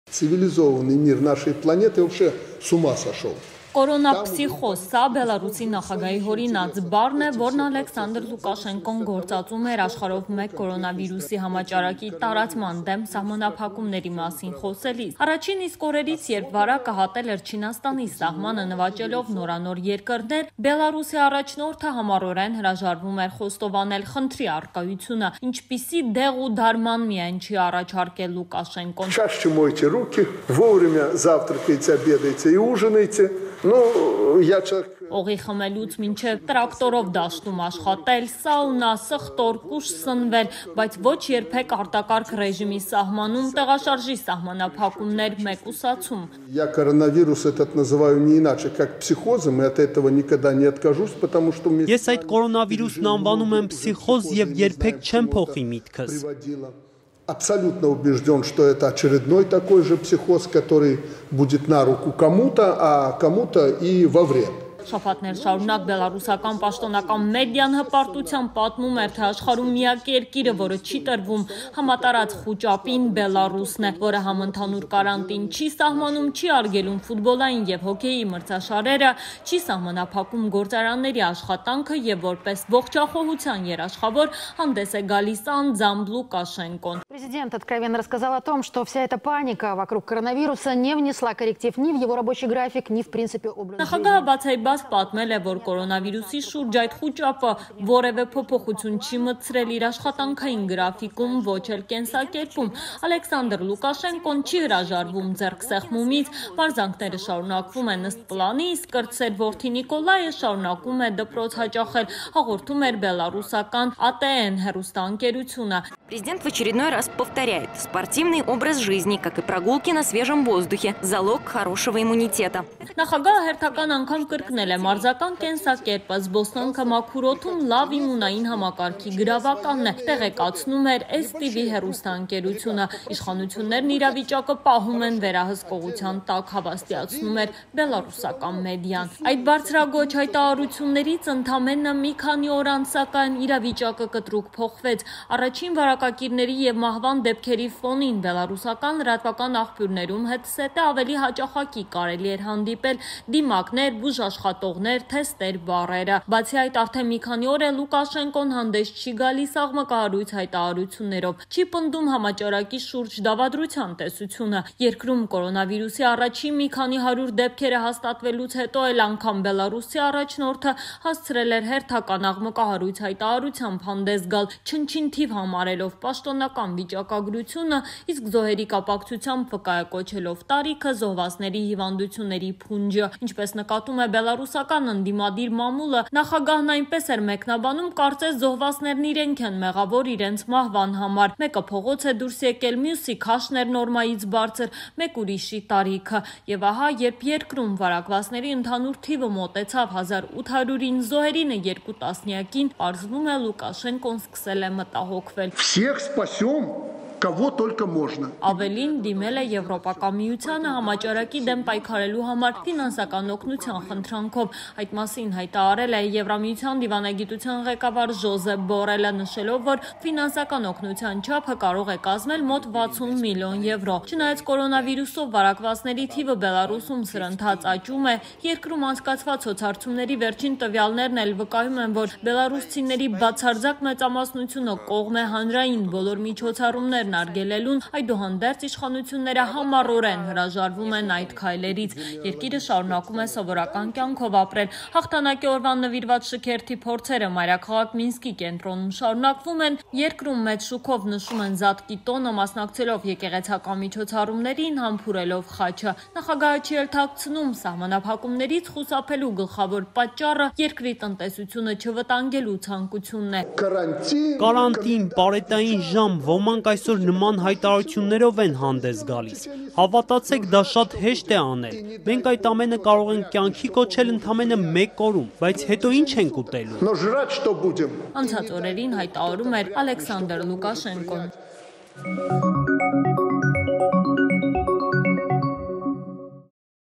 Արդեն մի քանի օր է՝ Լուկաշենկոն կորոնավիրուսի մասին սկանդալային հայտարարություններ չի անում
«Կորոնափսիխոզ»` սա Բելառուսի նախագահի հորինած բառն է, որն Ալեքսանդր Լուկաշենկոն գործածում էր աշխարհով մեկ կորոնավիրուսի համաճարակի տարածման դեմ սահմանափակումների մասին խոսելիս: